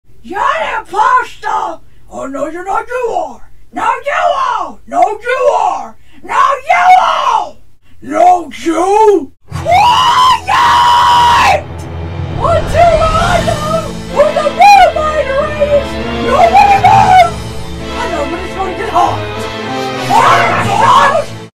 The Imposter (Comic Dub)
When Mater discovers Lord Hater has disguised himself as him, it begins a fight between them to prove who is the imposter and who’s not.